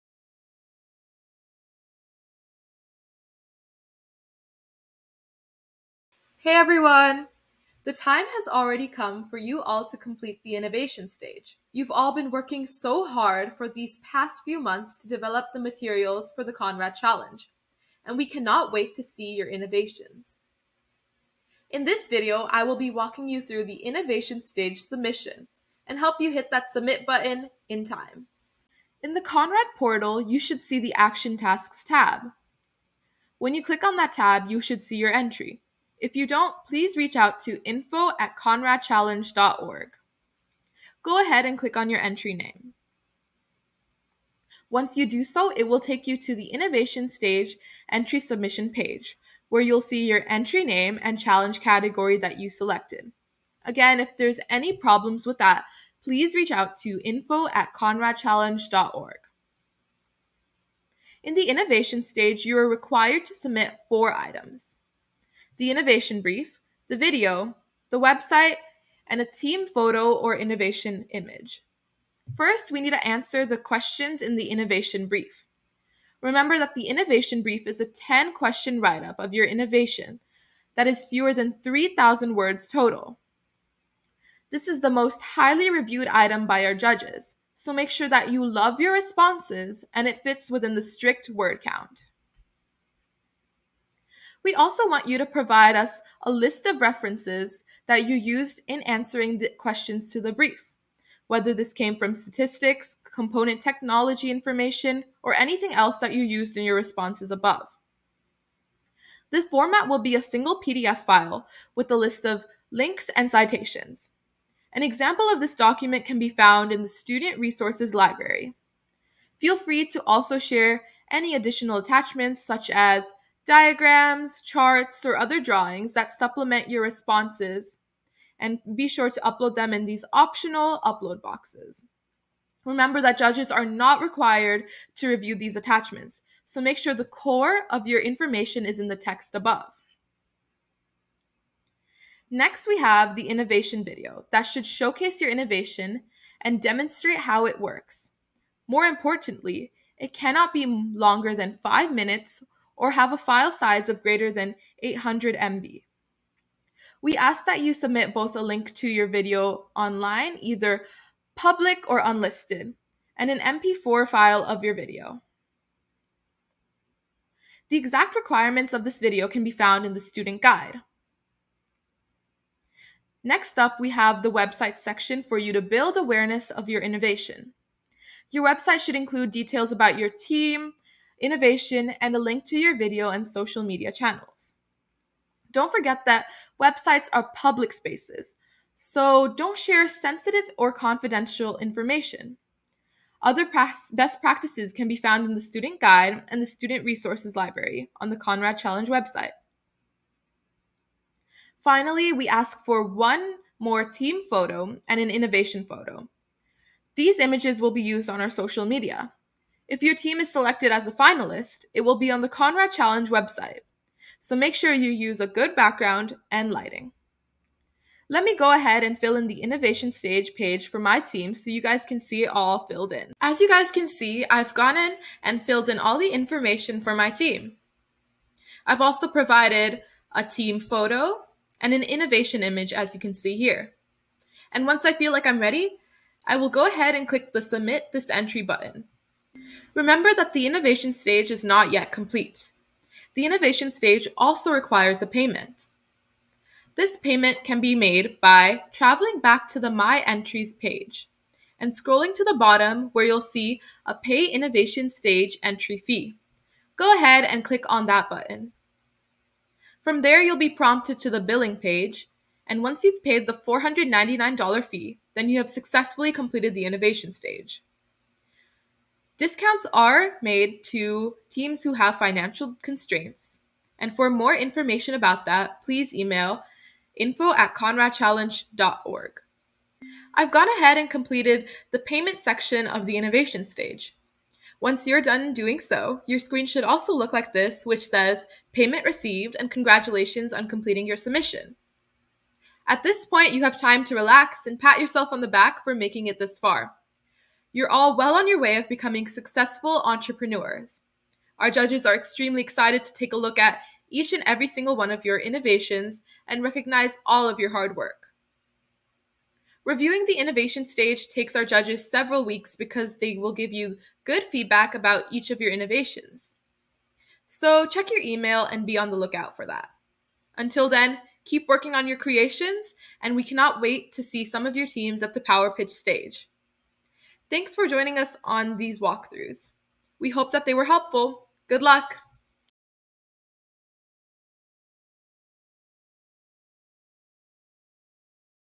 2023-24 Conrad Challenge | Innovation Stage Walk-Through
During the Innovation Stage of the 2023-24 Conrad Challenge, you will create an Innovation Brief, Innovation Video and a website! You’ll share more about your team, how your innovation works, the impact you intend to have, and how you’ll spread your idea to the rest of the world. By completing this stage, you will receive feedback from industry professionals and be named a Conrad Innovator.